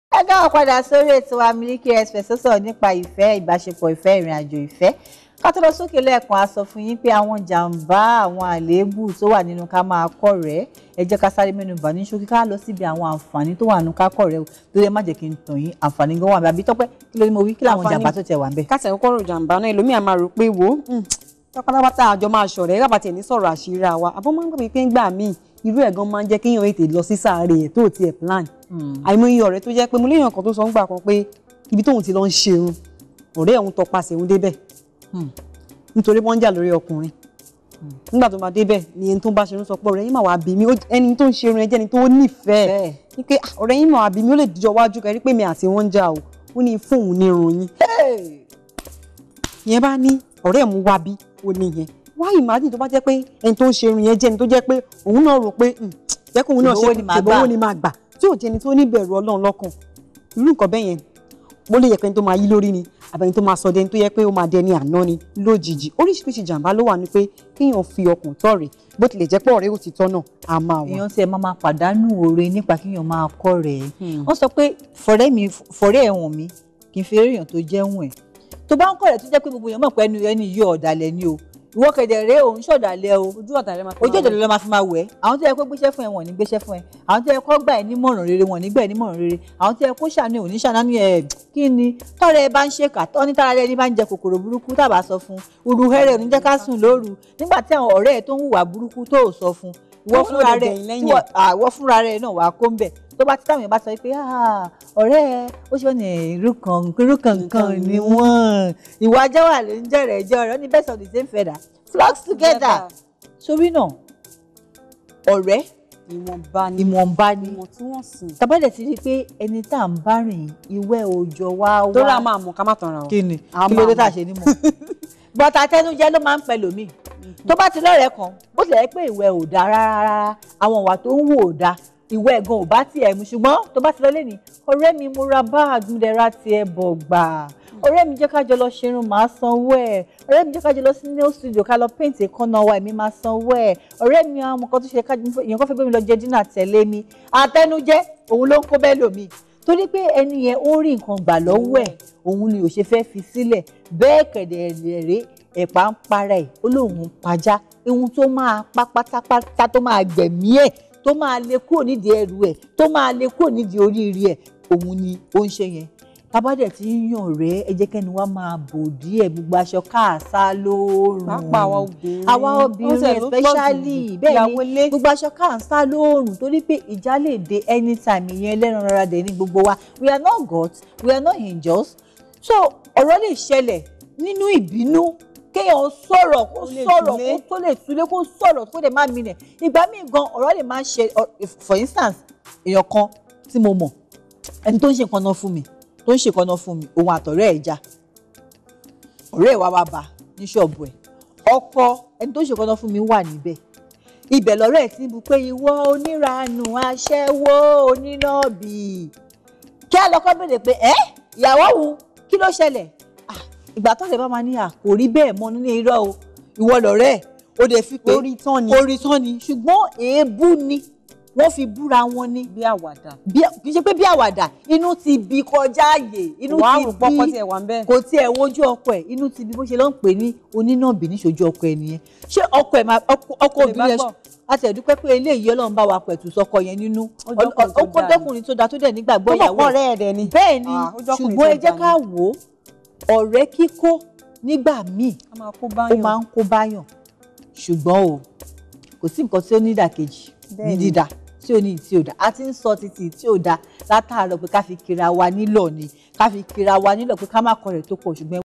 MILIKI EXPRESS: KII SE GBOGBO ORO LA LE SO FON ORE AGAGA AWON OBIRIN
Gege bi a ti mo pe kii se gbogbo aso laa n sa si orun bee gege nooni kii se gbogbo oro la n so fun ore.